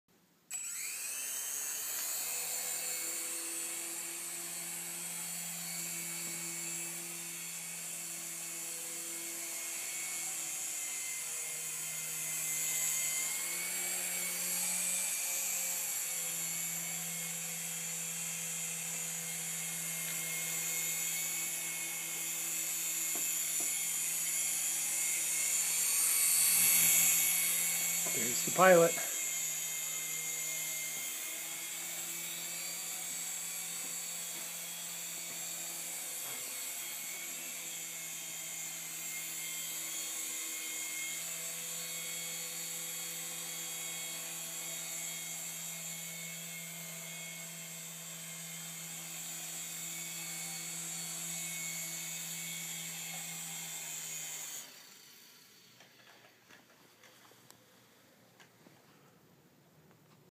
Alex and his Christmas present
Alex and his Christmas present, an IMEX MIC1200 "Shark" RC helicopter.